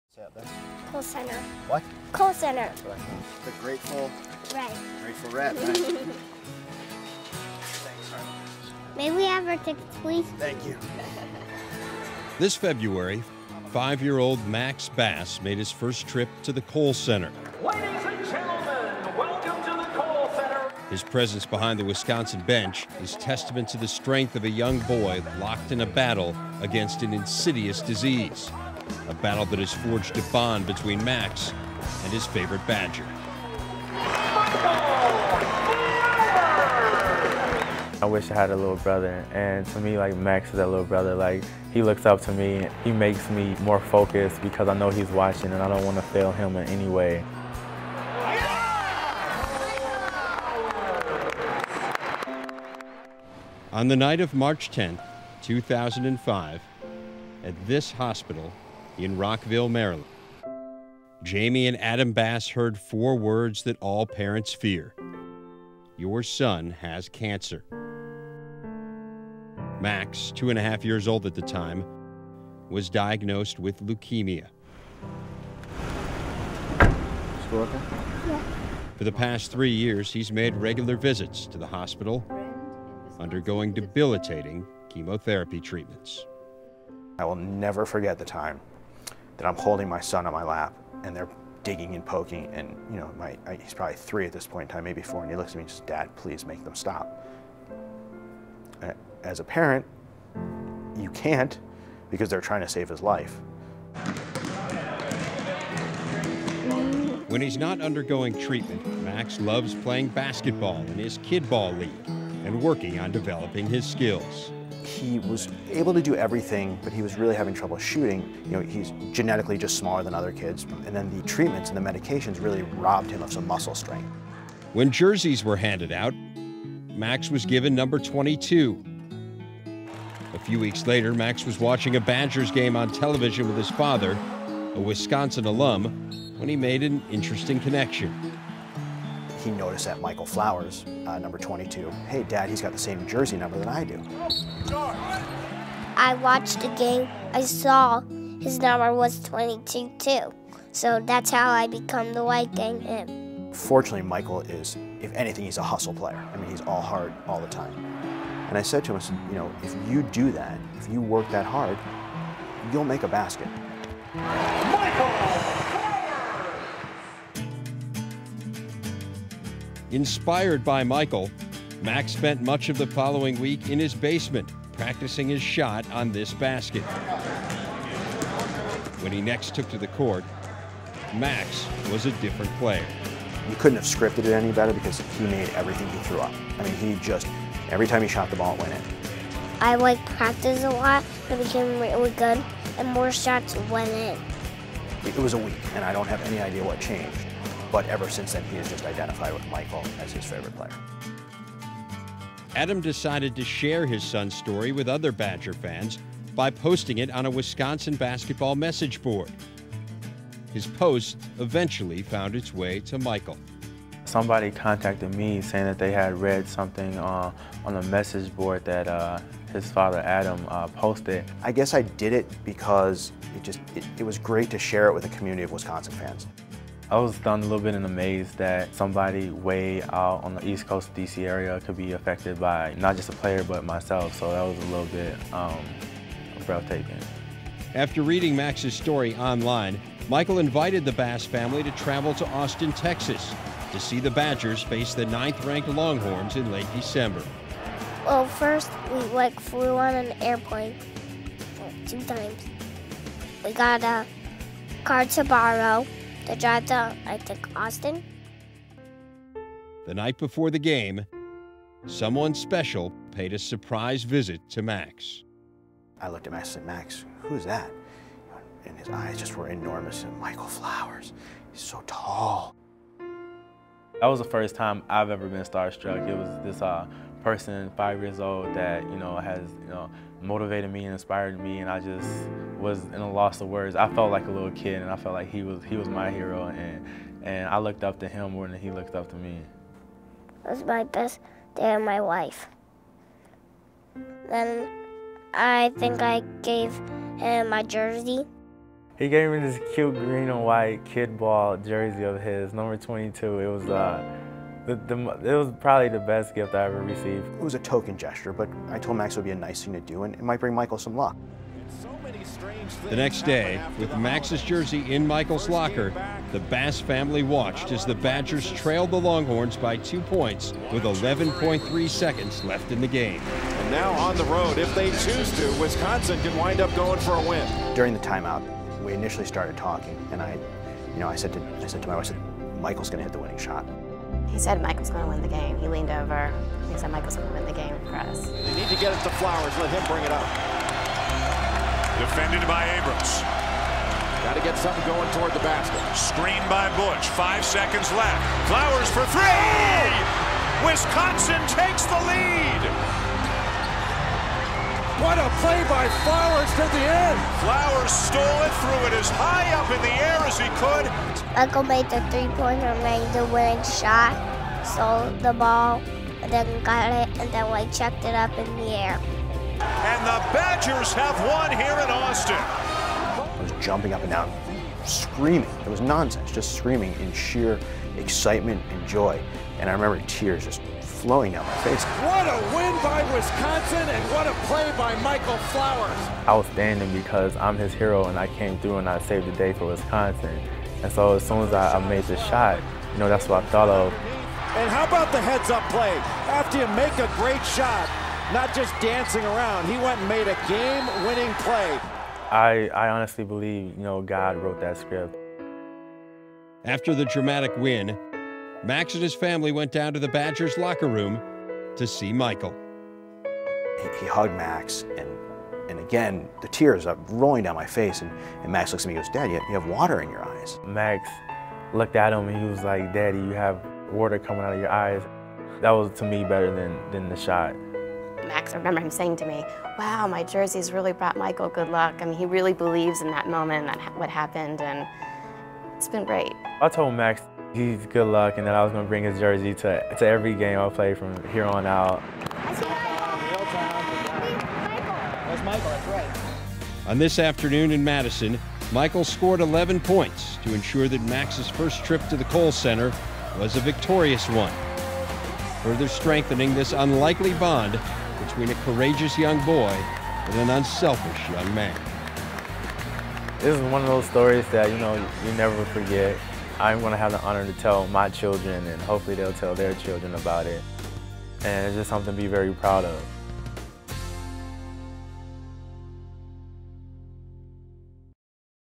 Wisconsin's Michael Flowers and his special fan Max
Wisconsin Guard Michael Flowers was inspired after hearing about 5-year old Max Bass' battle with Leukemia and found out the two shared a common bond.